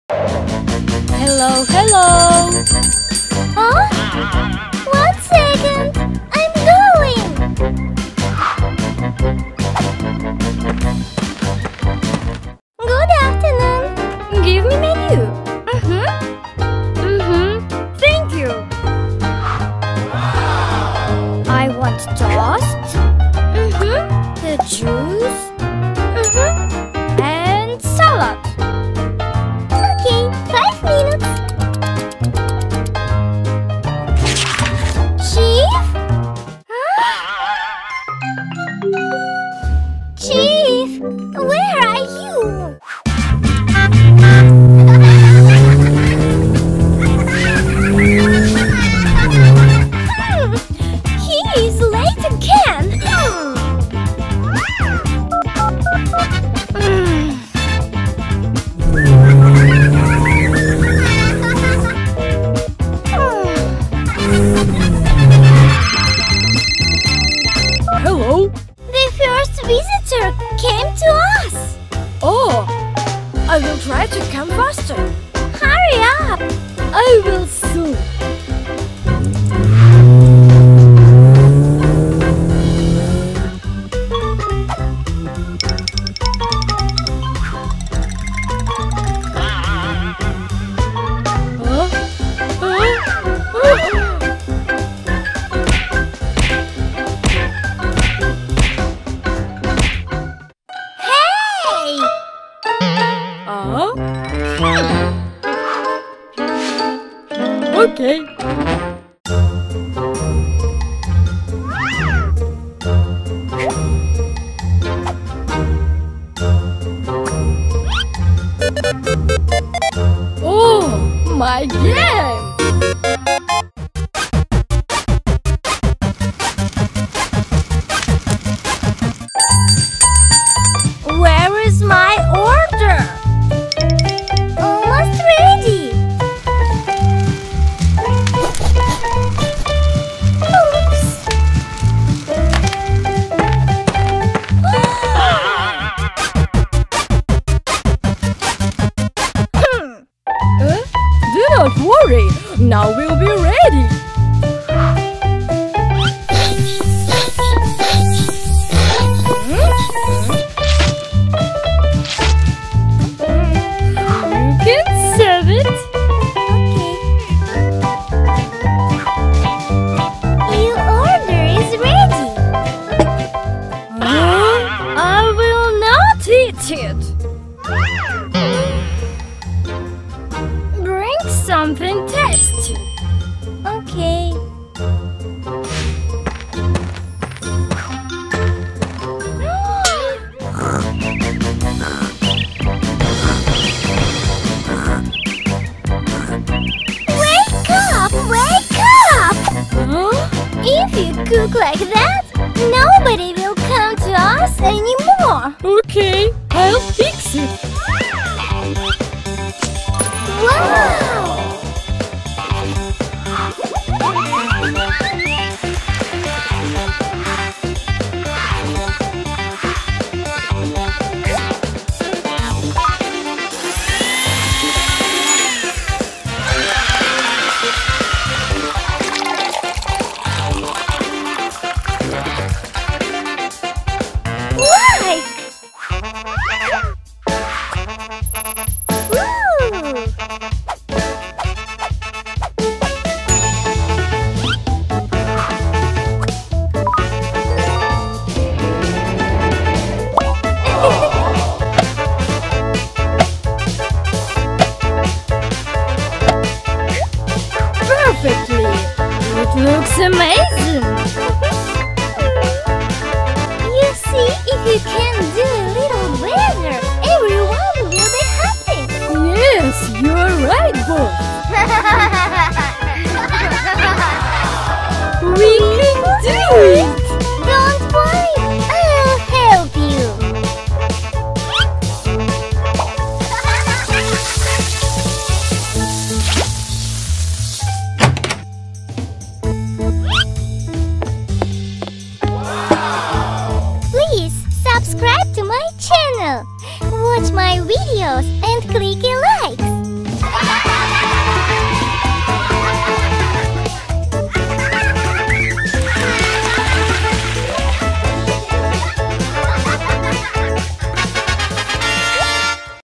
Alice and papa play in Restaurant with Kids Toys
Alice Smile and papa play in Restaurant with Kitchen Play Set Toys. Dady Pretend chef and makes delicious meals. Alice helps papa to Cooking.